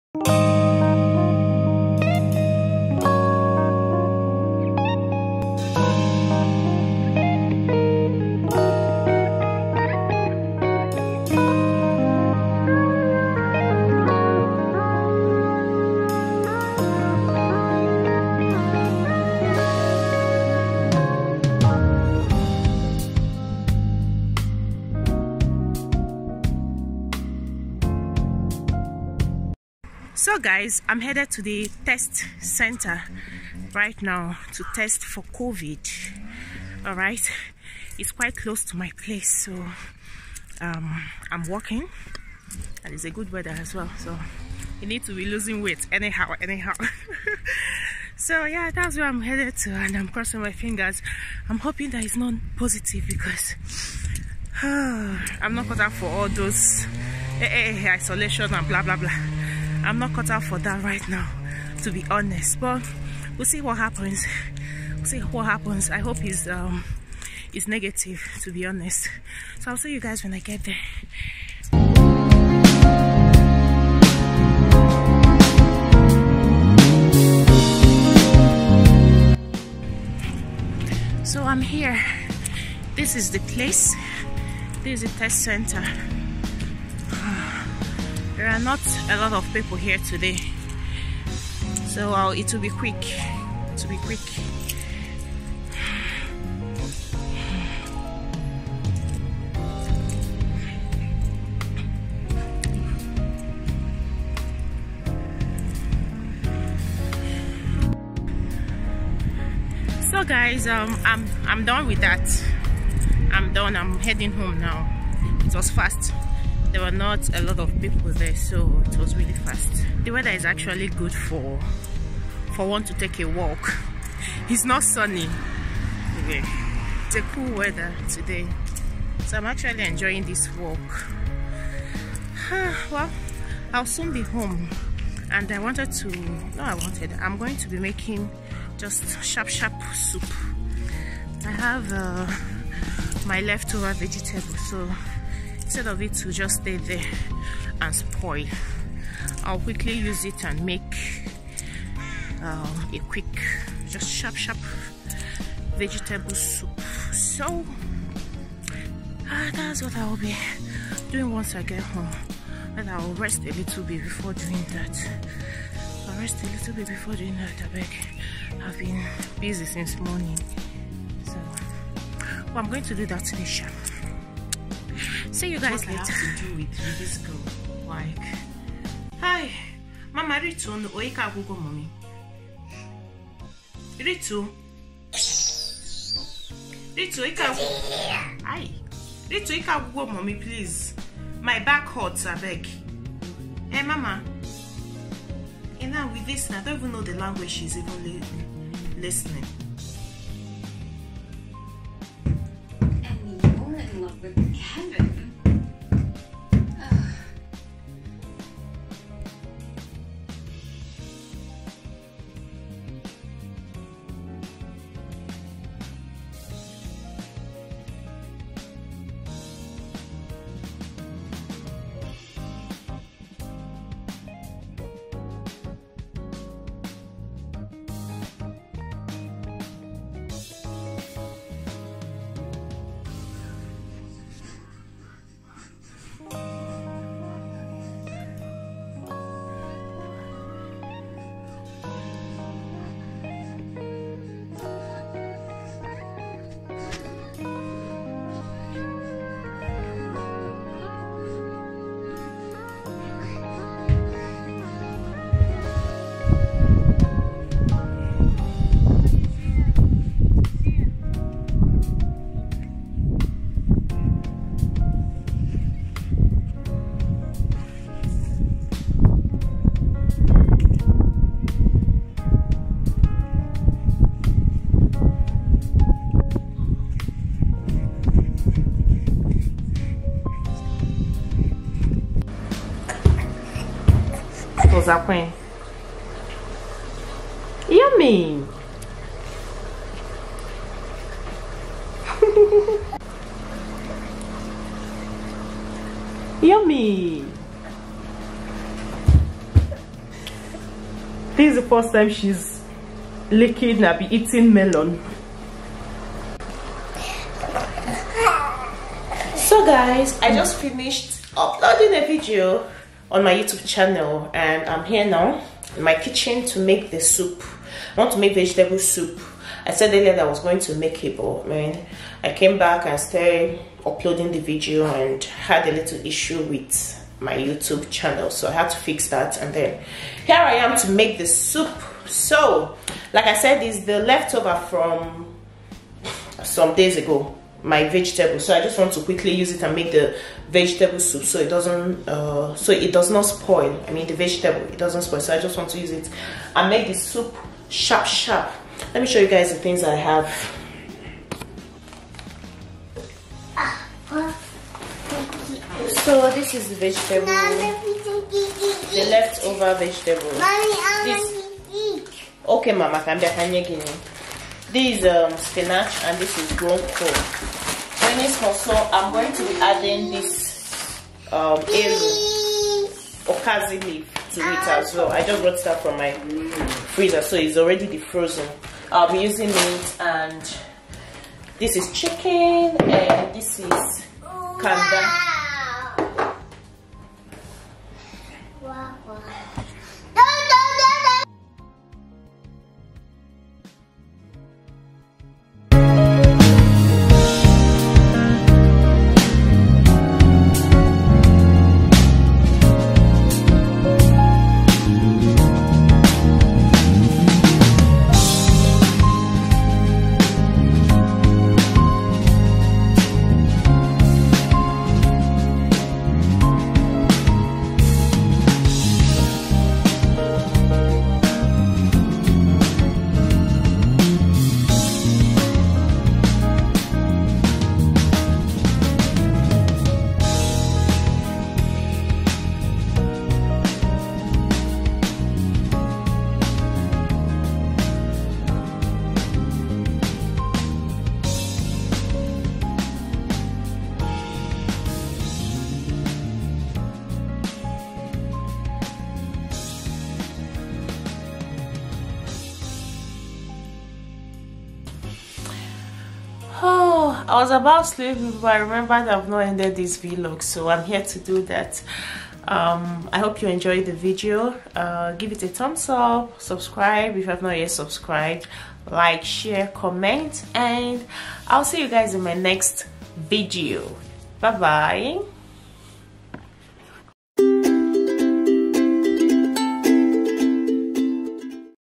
I TESTED Again, My 2 Year Old REACTION to Eating LEMON For The First Time, Cooking, Life In Denmark
Please share if you enjoyed the video ❤️